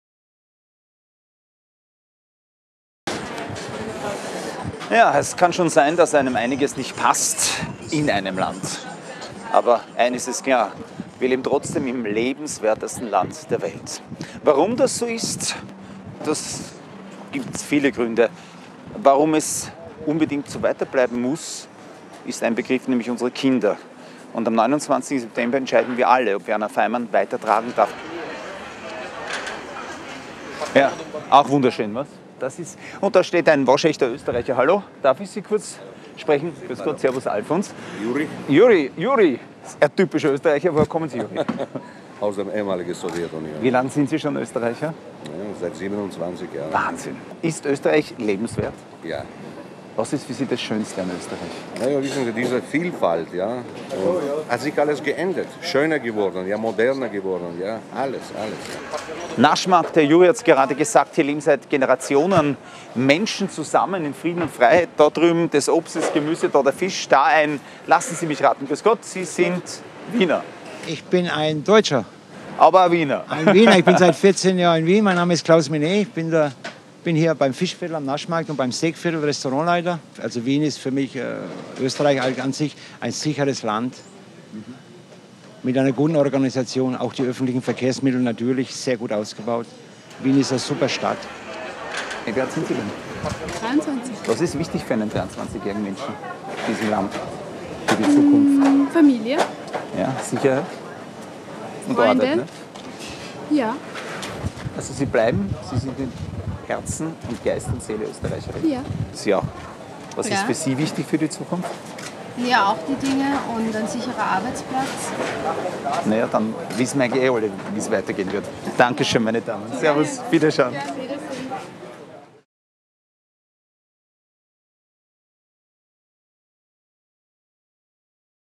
Alfons Haider am Naschmarkt
Alfons Haider trifft am Wiener Naschmarkt Menschen und spricht mit ihnen über Österreich.